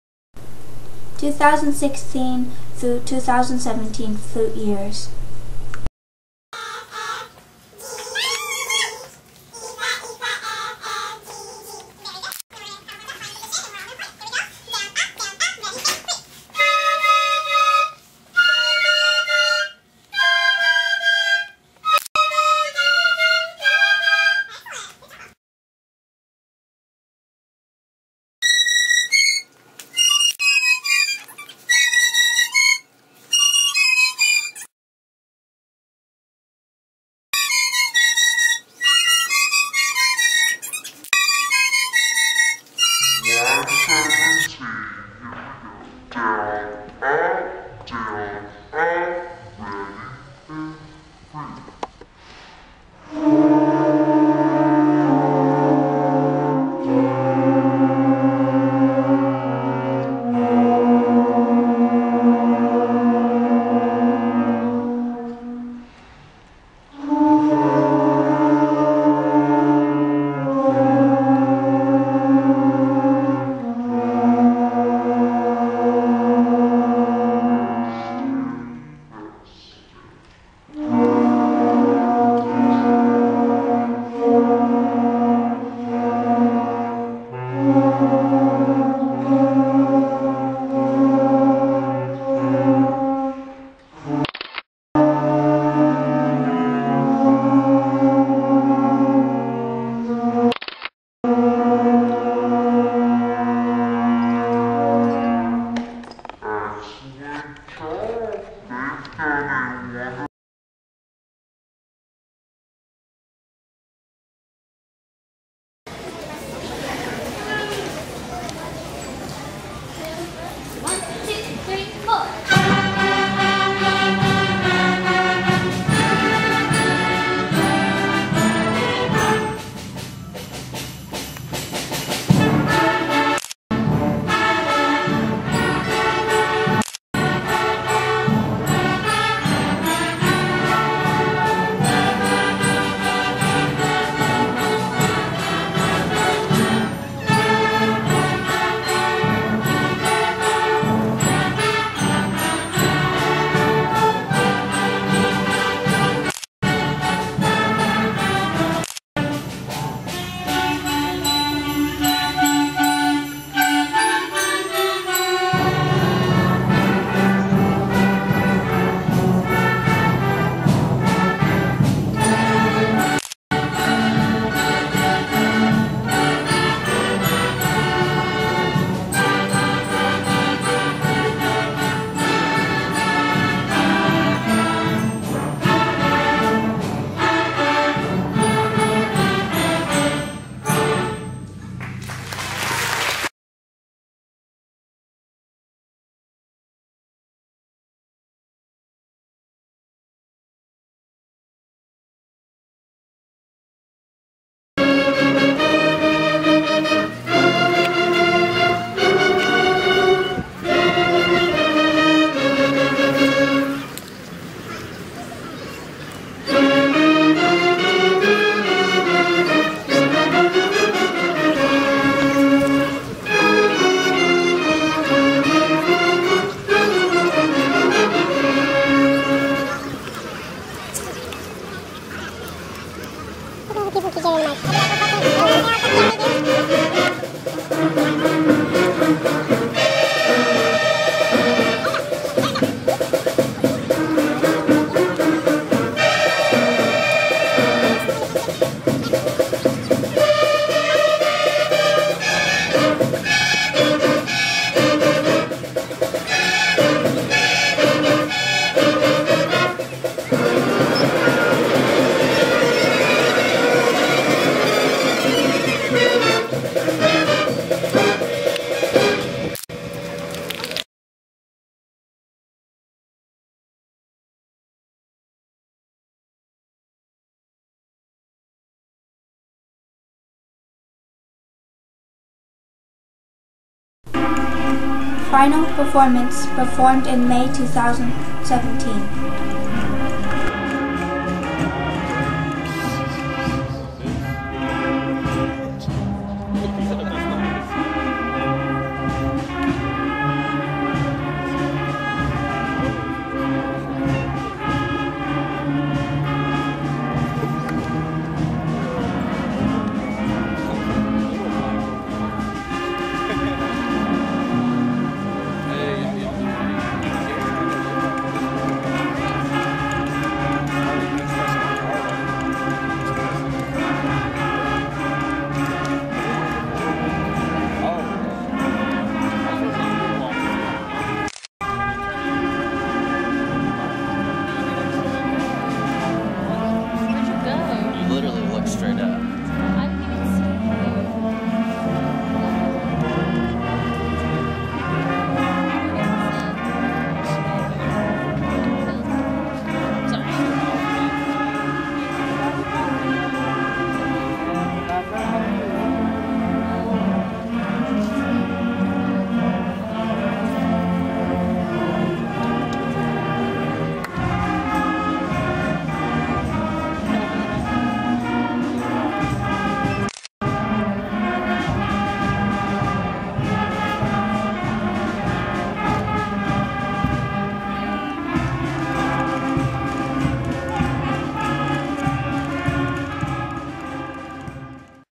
2017--2016 FLUTE YEARS
ALICE1BLUE VIDEO
supporting the Music
daughter playing  flute thur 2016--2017 mesh clips